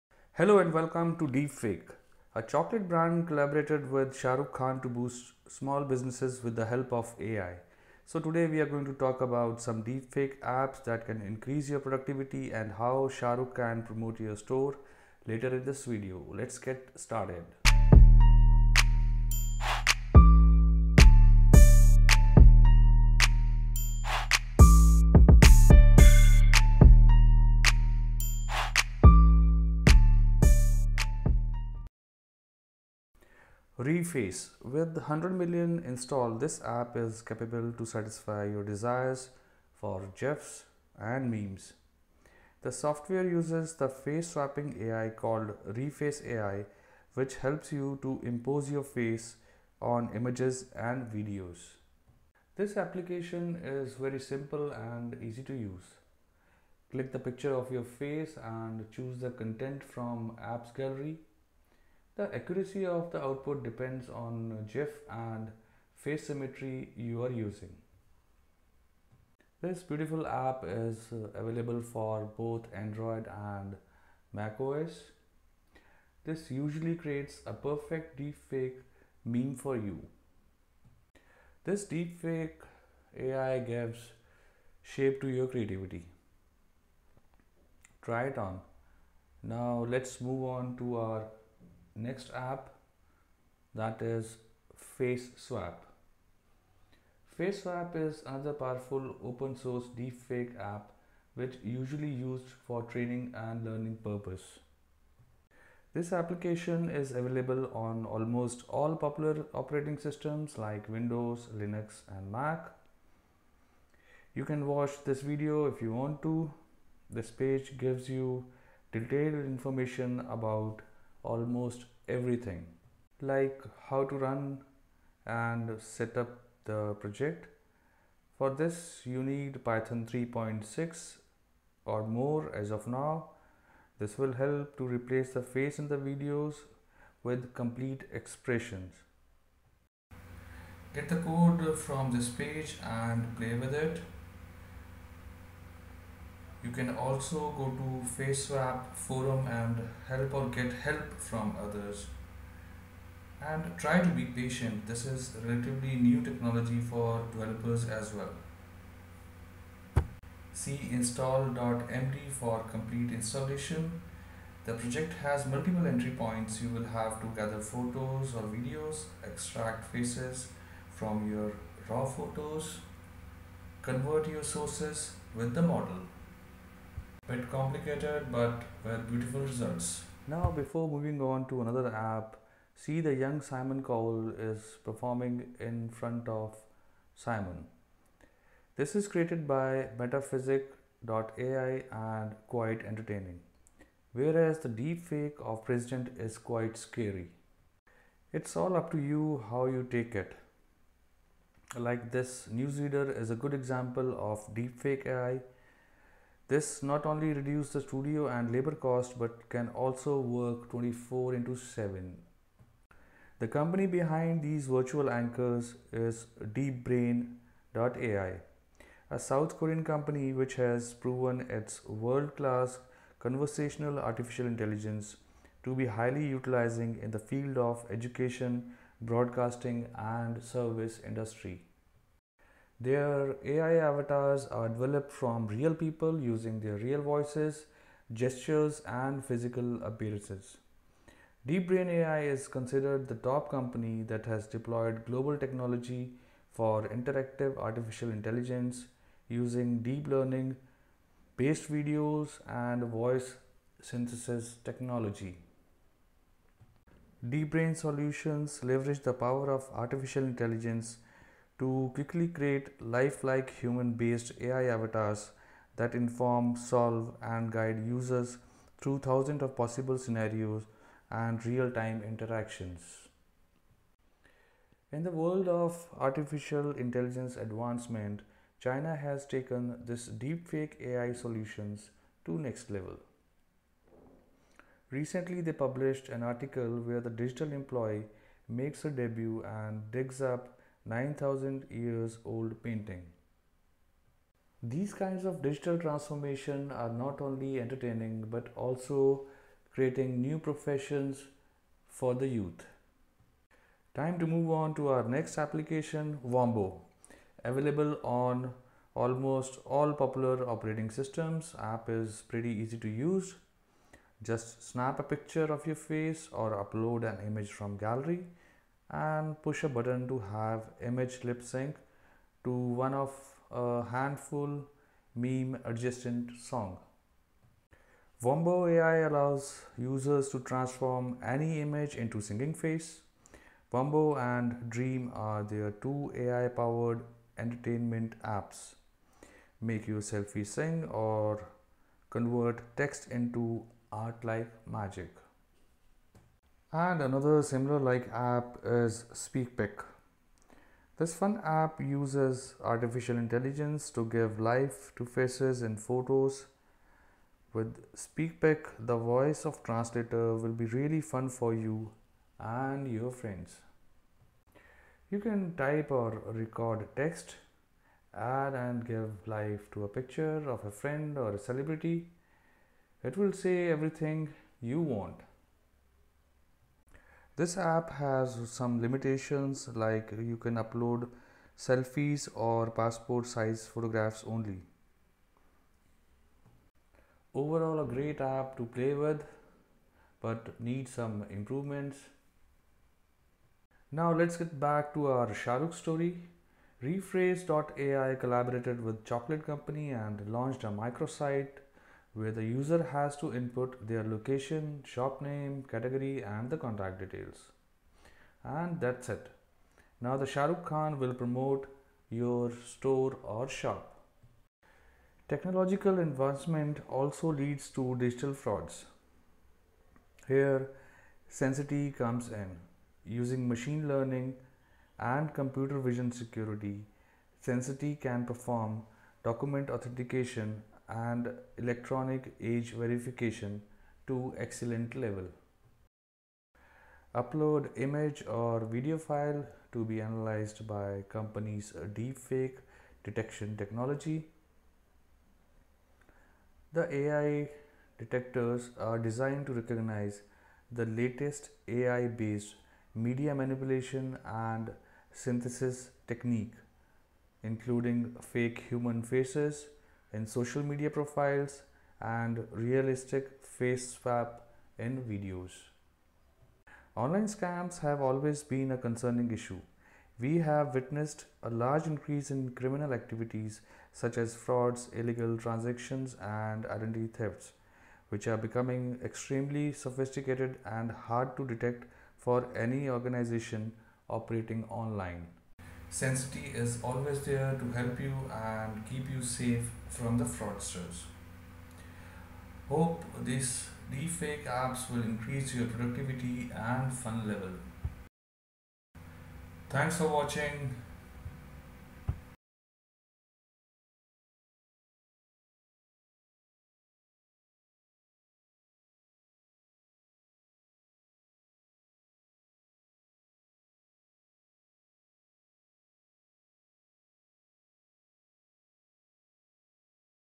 Deepfake | A I
The advancement of deepfake technology have positive impacts. In fact, there are some interesting cases to be made for the implementation of deep learning AI in media industries, the medical field, the technological field, or the improvement of user interfaces. In this video we tried figure out some applications that will increase productivity and fun level. Get ready to roll.